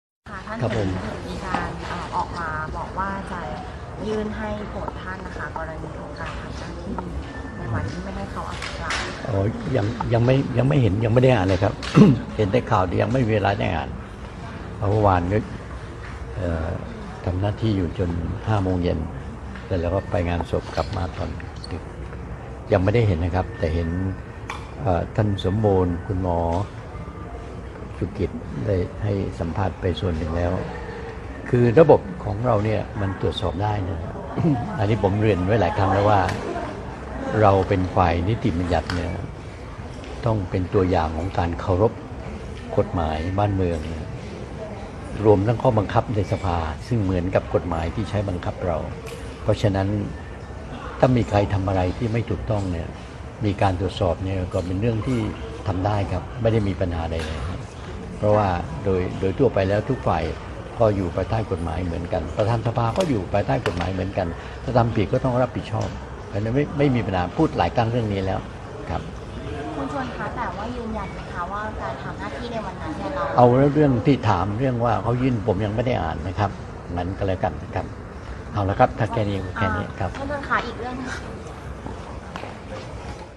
“ชวน “พร้อมให้ “เสรีพิศุทธิ์” ตรวจสอบหากเห็นว่าไม่ถูกต้อง ตามระบบรัฐสภา
22/0/2019 “ชวน “พร้อมให้ “เสรีพิศุทธิ์” ตรวจสอบหากเห็นว่าไม่ถูกต้อง ตามระบบรัฐสภา
 "ประธานสภาผู้แทนราษฎร" เมิน "เสรีพิศุทธ์" ตะเพิดอ้างทำขัด รธน. ยืนยันระบบรัฐสภาตรวจสอบได้ ย้ำเป็นฝ่ายนิติบัญญัติต้องเป็นตัวอย่างของการเคารพกฎหมายบ้านเมือง รวมถึงข้อบังคับในสภาที่เหมือนกฎหมายที่ใช้บังคับ ส.ส.ด้วย หากมีใครทำอะไรที่ไม่ถูกต้อง และมีการตรวจสอบถือว่าเป็นเรื่องที่ทำได้